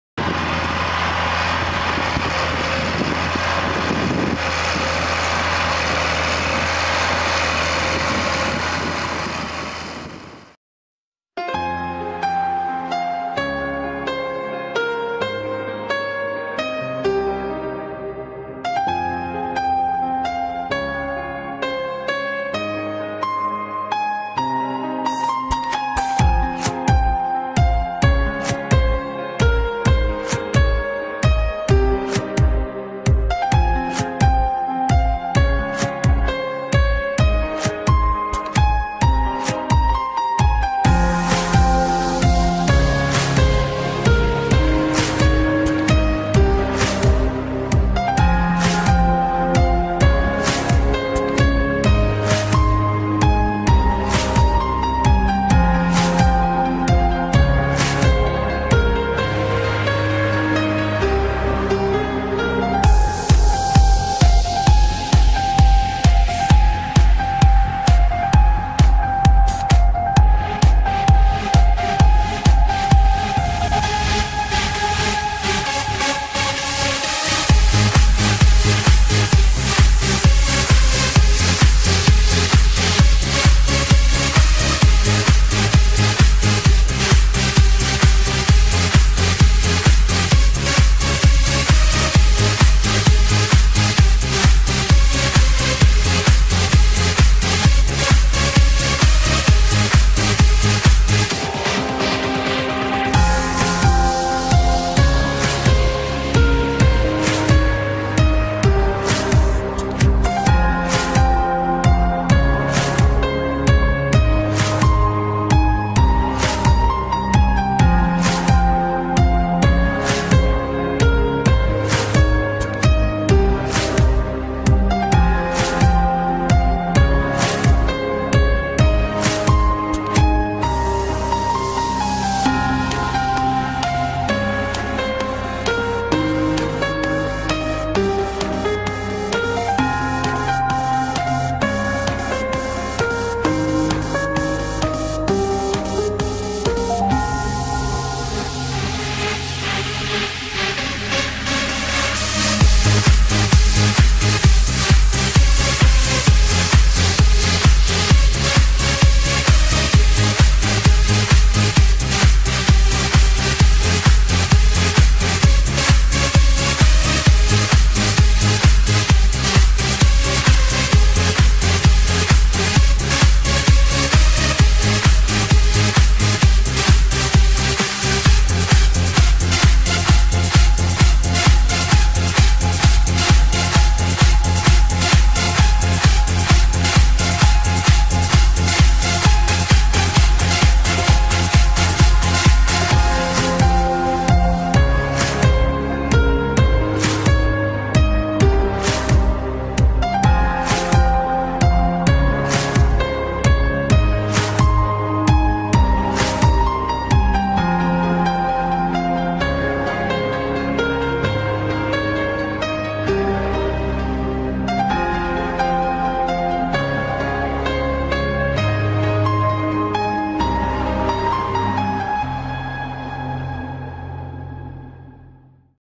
Wiosna 2013-Agregatowanie i Gruberowanie
Witam zapraszam do subskrypcji
Ursus 1614-gruber 3m
Ford 8630-agregat 5,20m